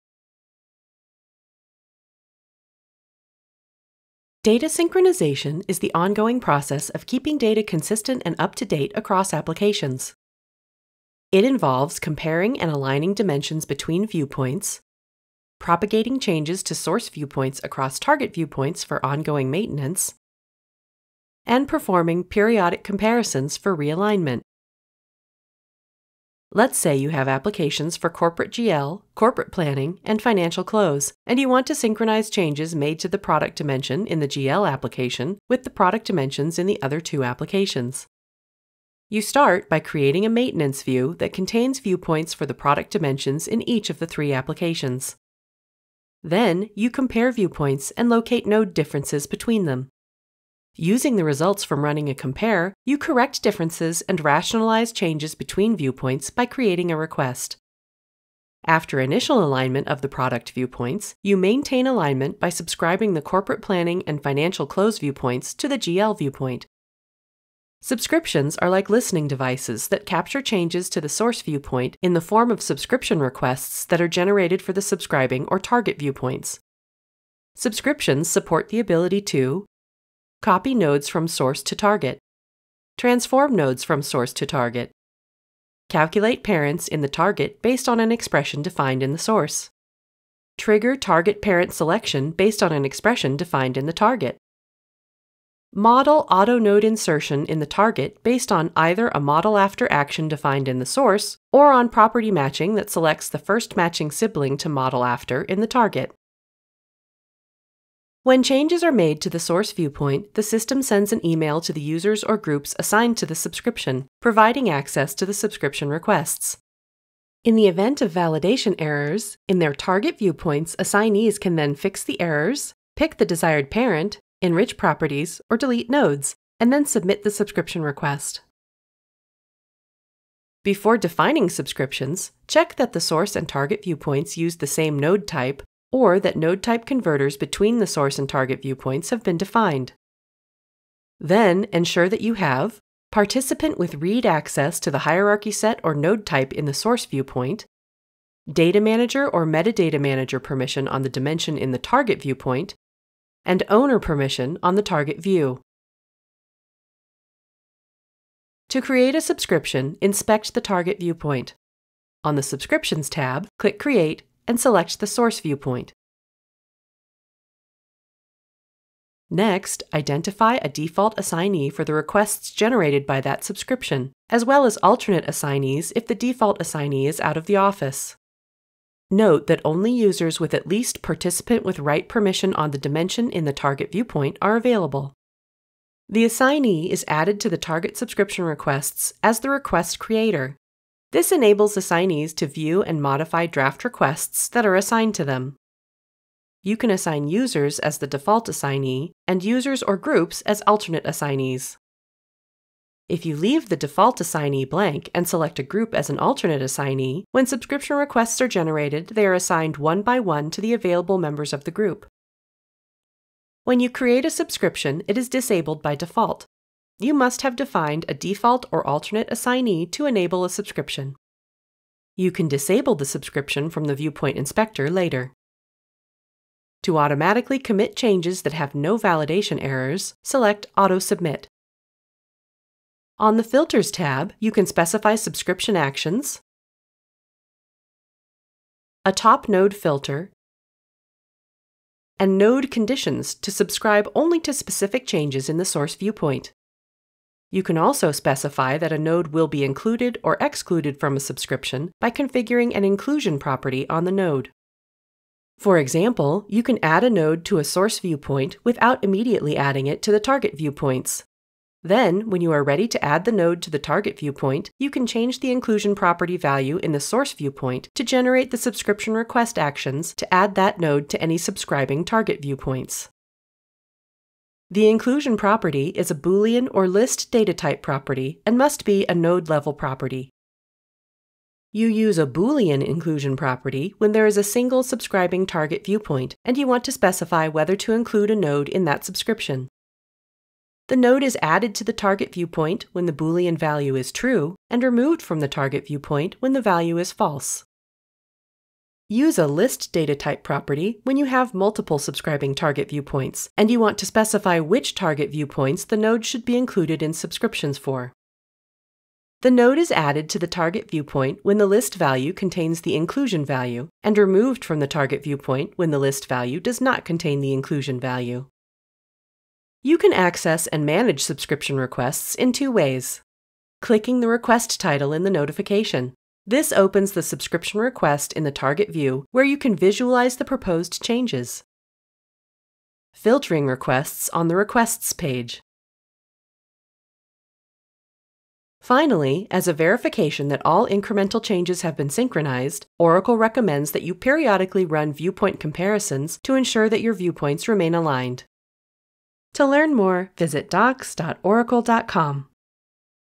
Synchronizing Data Changes in Oracle Enterprise Data Management
This overview describes how subscriptions let you automate change management by propagating source dimension changes across different business perspectives to ensure predictability and consistency.

Timings:
0:05 Data synchronization process
0:26 Data synchronization example
0:39 Initial alignment
0:59 Ongoing maintenance using subscriptions
1:07 Subscriptions
1:48 The subscription process
2:10 Prerequisites
2:38 Creating subscriptions
4:13 Inclusion properties
5:19 Accessing subscription requests